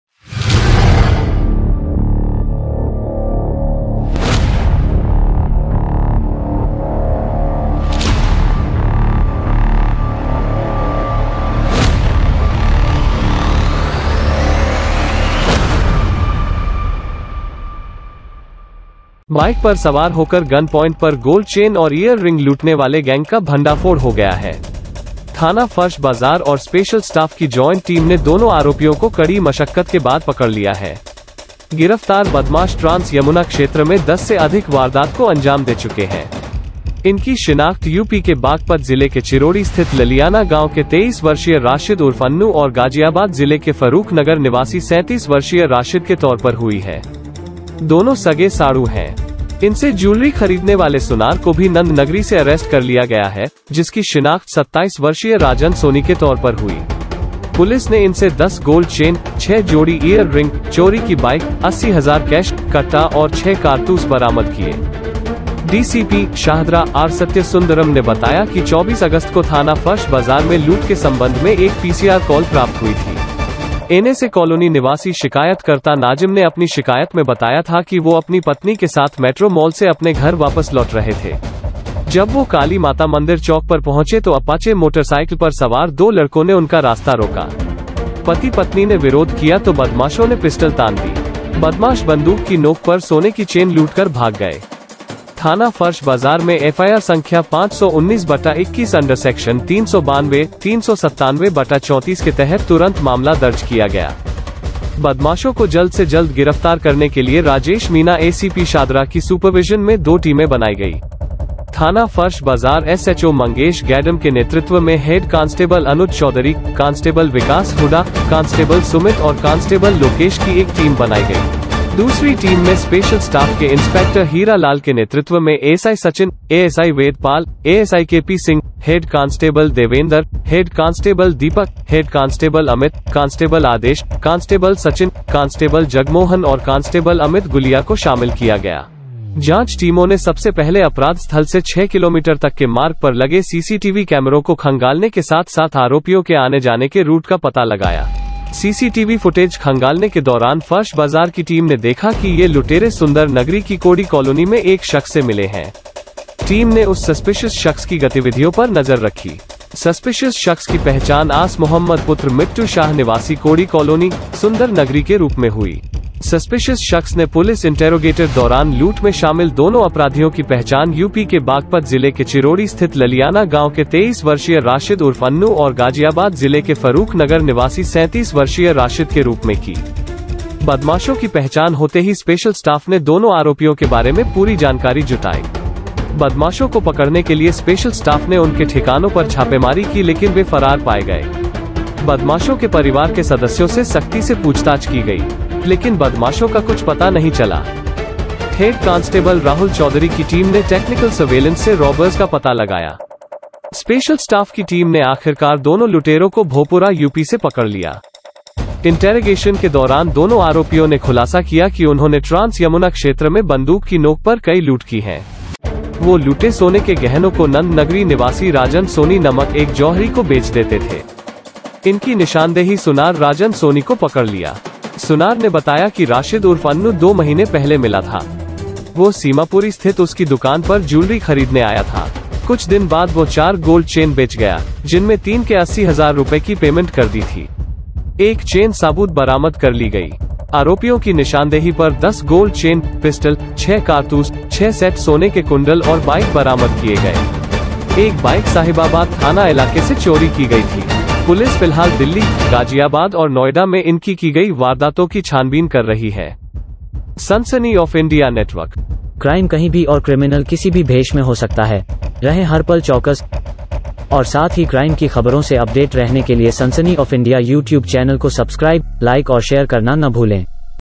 गन पॉइंट पर लूट करने वाले दो सगे साढ़ू गिरफ्तार
बाइक पर सवार होकर गन पॉइंट पर गोल्ड चेन और ईयर रिंग लूटने वाले गैंग का भंडाफोड़ हो गया है। थाना फरश बाजार और स्पेशल स्टाफ की जॉइंट टीम ने  दोनों आरोपियों को कड़ी मशक्कत के बाद पकड़ लिया है। गिरफ्तार बदमाश ट्रांस-यमुना क्षेत्र में 10 से अधिक वारदात को अंजाम दे चुके हैं। इनकी शिनाख्त यूपी के बागपत जिले के चिरोड़ी स्थित ललियाना गांव के 23 वर्षीय  राशिद उर्फ अन्नू और गाजियाबाद जिले के फरूख नगर निवासी 37 वर्षीय राशिद के तौर पर हुई है। दोनों सगे साढ़ूू हैं।
इनसे जूलरी खरीदने वाले सुनार को भी नंद नगरी से अरेस्ट कर लिया गया है, जिसकी शिनाख्त 27 वर्षीय राजन सोनी के तौर पर हुई। पुलिस ने इनसे 10 गोल्ड चेन, 6 जोड़ी ईयर रिंग, चोरी की बाइक, 80 हजार कैश, कट्टा और 6 कारतूस बरामद किए।

डीसीपी (शाहदरा) आर सत्यसुंदरम ने बताया कि 24 अगस्त को थाना फरश बाजार में लूट के संबंध में एक पीसीआर कॉल प्राप्त हुई थी। एनएसए कॉलोनी निवासी शिकायतकर्ता  नाजिम ने अपनी शिकायत में बताया था की वो अपनी पत्नी के साथ मेट्रो मॉल से अपने घर वापस लौट रहे थे। जब वो काली माता मंदिर चौक पर पहुंचे तो  अपाचे मोटरसाइकिल पर सवार दो लड़कों ने उनका  रास्ता रोका। पति-पत्नी ने विरोध किया तो बदमाशों ने पिस्टल तान दी। बदमाश बंदूक की नोक पर सोने की चेन लूट कर भाग गए। थाना फरश बाजार में एफआईआर संख्या 519/21 अंडर सेक्शन 392/397/34 के तहत तुरंत मामला दर्ज किया गया।

बदमाशों को जल्द से जल्द  गिरफ्तार करने के लिए Rajesh Meena ACP Shahdara की supervision में दो टीमें बनाई गई । थाना फरश बाजार एसएचओ मंगेश गेडम के नेतृत्व में हेड कांस्टेबल अनुज चौधरी, कांस्टेबल विकास हुडा, कांस्टेबल सुमित और कांस्टेबल लोकेश की एक टीम बनाई गई।
दूसरी टीम में स्पेशल स्टाफ के इंस्पेक्टर हीरा लाल के नेतृत्व में एसआई सचिन, एएसआई वेद पाल, एएसआई केपी  सिंह, हेड कांस्टेबल देवेंदर, हेड कांस्टेबल दीपक, हेड कांस्टेबल अमित,  कांस्टेबल आदेश, कांस्टेबल सचिन, कांस्टेबल जगमोहन और कांस्टेबल अमित  गुलिया  को शामिल किया  गया।

जांच टीमों ने सबसे पहले अपराध स्थल से 6 किलोमीटर तक के मार्ग पर लगे सीसीटीवी कैमरों को खंगालने के साथ साथ आरोपियों के आने-जाने के रूट का पता लगाया।
सीसीटीवी फुटेज खंगालने  के दौरान फरश बाजार की टीम ने देखा कि ये लुटेरे Sunder Nagri की कोढ़ी कॉलोनी में एक शख्स से मिले हैं। टीम ने उस suspicious शख्स की गतिविधियों पर नजर रखी। suspicious शख्स की पहचान आस मोहम्मद पुत्र मिट्टू शाह निवासी कोढ़ी कॉलोनी, सुंदर नगरी के रूप में हुई।
suspicious शख्स ने पुलिस interrogated दौरान  लूट में शामिल दोनों अपराधियों की पहचान यूपी के बागपत जिले के चिरोड़ी स्थित ललियाना गांव के 23 वर्षीय  राशिद उर्फ अन्नू और गाजियाबाद जिले के फरूख नगर निवासी 37 वर्षीय राशिद    के  रूप में की।

बदमाशों की पहचान होते ही स्पेशल स्टाफ ने दोनों आरोपियों के बारे में पूरी जानकारी जुटाई। बदमाशों को पकड़ने के लिए स्पेशल स्टाफ ने उनके ठिकानों पर छापेमारी की लेकिन वे फरार पाए गए। बदमाशों के परिवार के सदस्यों से सख्ती से पूछताछ की गई। लेकिन बदमाशों का कुछ पता नहीं चला।

हेड कांस्टेबल राहुल चौधरी की टीम ने  technical Surveillance से robbers का पता लगाया। स्पेशल स्टाफ की टीम ने आखिरकार दोनों लुटेरों को भोपुरा यूपी से पकड़ लिया।

interrogation के दौरान दोनों आरोपियों ने खुलासा किया कि उन्होंने ट्रांस यमुना क्षेत्र में बंदूक की नोक पर कई लूट की हैं। वो लूटे सोने के गहनों को नंद नगरी निवासी राजन सोनी नमक एक जौहरी को बेच देते थे।

इनकी निशानदेही सुनार राजन सोनी को पकड़ लिया।  सुनार ने बताया कि राशिद उर्फ अन्नू दो महीने पहले मिला था। वह सीमापुरी स्थित उसकी दुकान पर जूलरी खरीदने आया था। कुछ दिन बाद वह चार गोल्ड चेन बेच गया, जिनमें तीन के 80 हजार रुपये की पेमंट कर दी थी। एक चेन साबूत बरामद कर ली गई। आरोपियों की निशानदेही पर 10 गोल्ड चेन, पिस्टल, 6 कारतूस, छह सेट सोने के कुंडल और बाइक बरामद किए गए। एक बाइक साहिबाबाद थाना इलाके से चोरी की गई थी। पुलिस फिलहाल दिल्ली, गाजियाबाद और नोएडा में इनकी की गई वारदातों की छानबीन कर रही है।